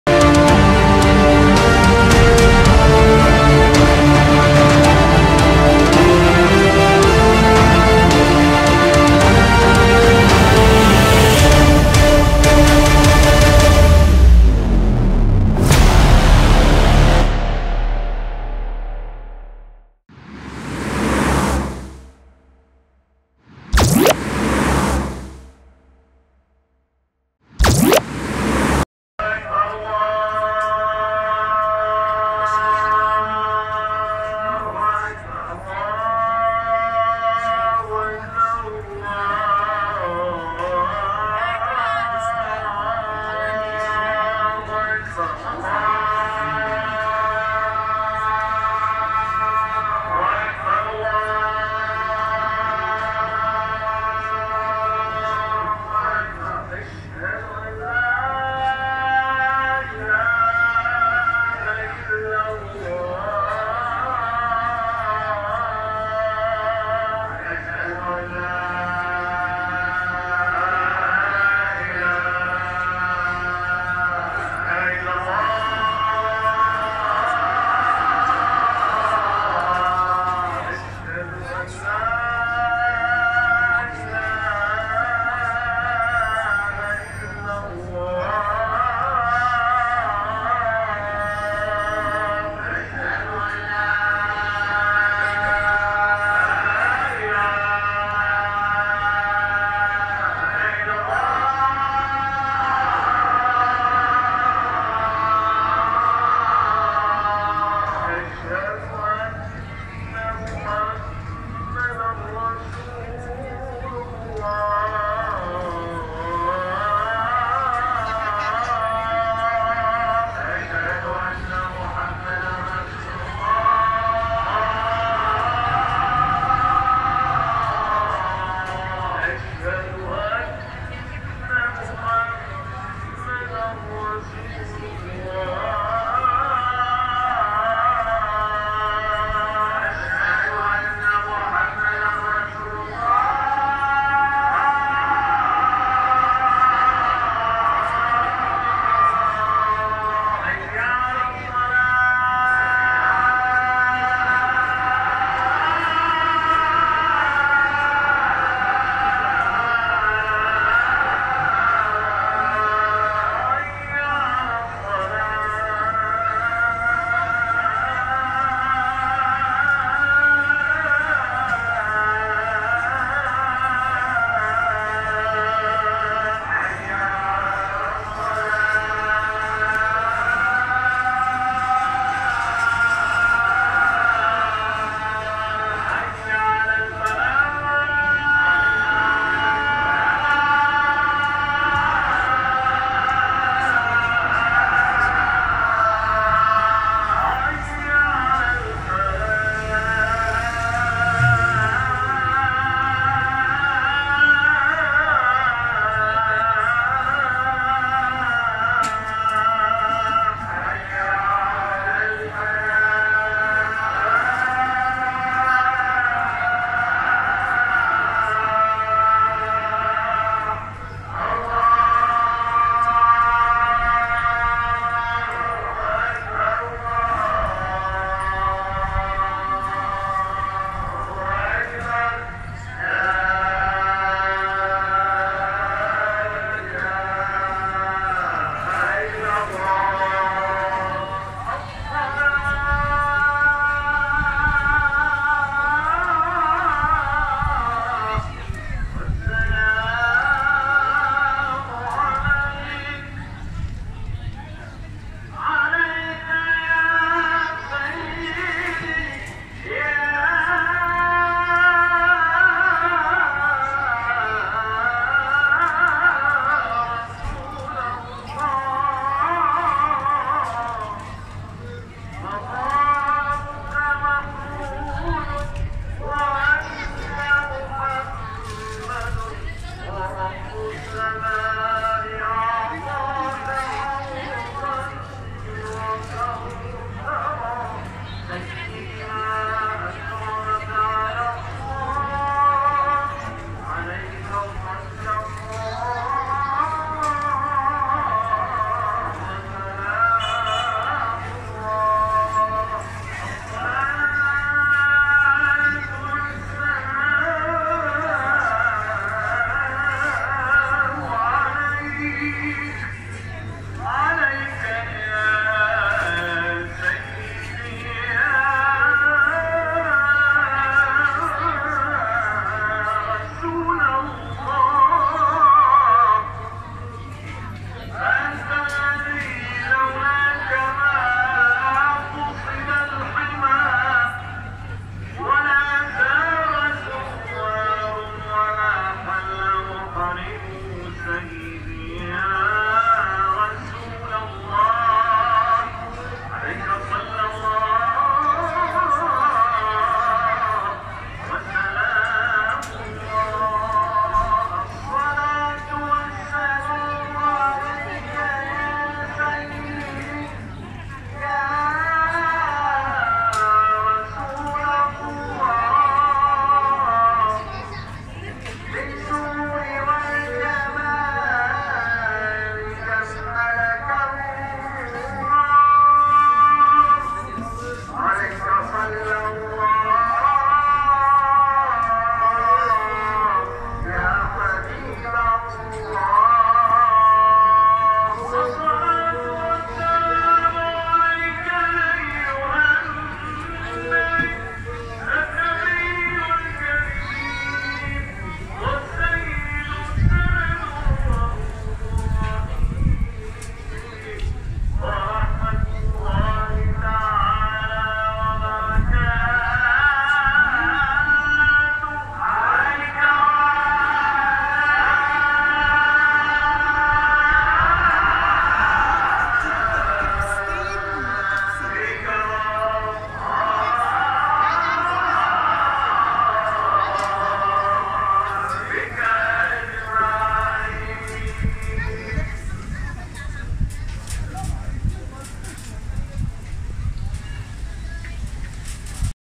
Call to Prayer - Umayyad Mosque, Damascus, Syria
The prayer call over the streets of old Damascus, Is not the typical sound of the Muslim call to prayer. It is a carefully preserved Byzantine tune.